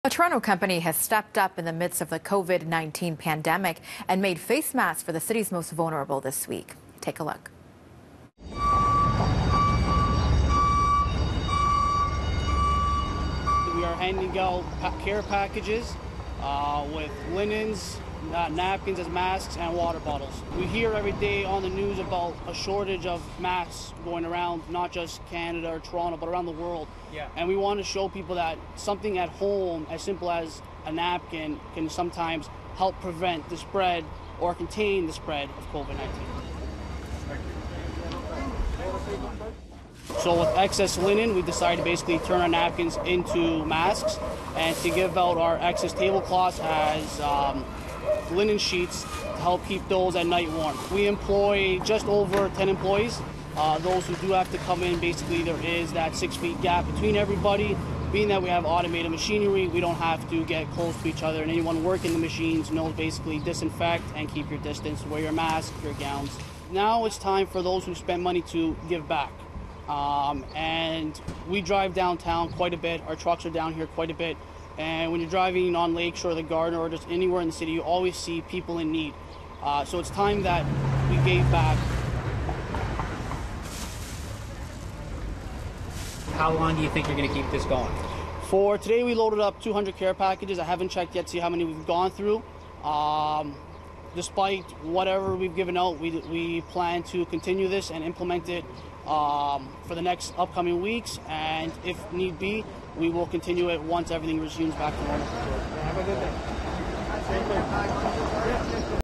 Toronto company makes facemasks for the homeless
A Toronto company is handing out masks to the city's most vulnerable to help protect them from COVID-19.